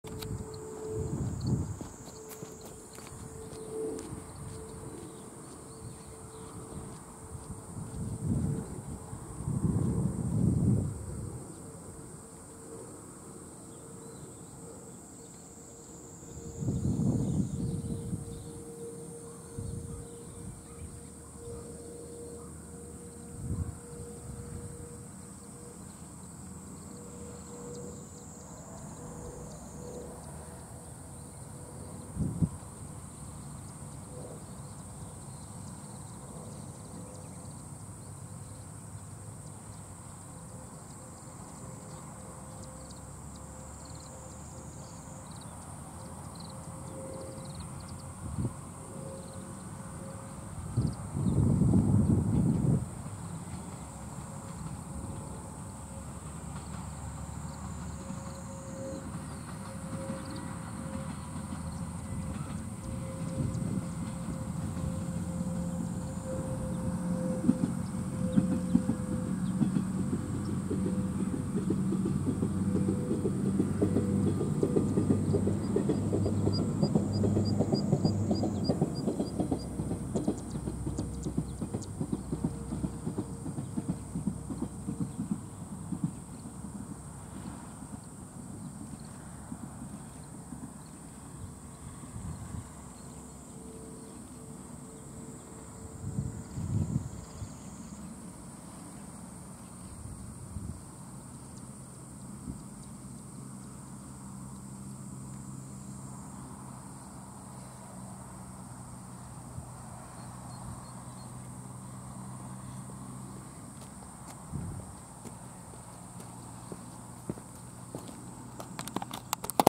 【天浜線】TH2108+TH2104 細谷～原谷通過
12号車がラッピング撤去される前、この2編成しかノーマル車が存在せず、たまたま撮影できた動画になります
少し違う場所で写真を撮影しましたが、久しぶりの白車重連に手振れが凄かったのを覚えています(笑)
12号車が復活したから少しは確立上がったかな？
また、走るようなら狙いたいです(笑)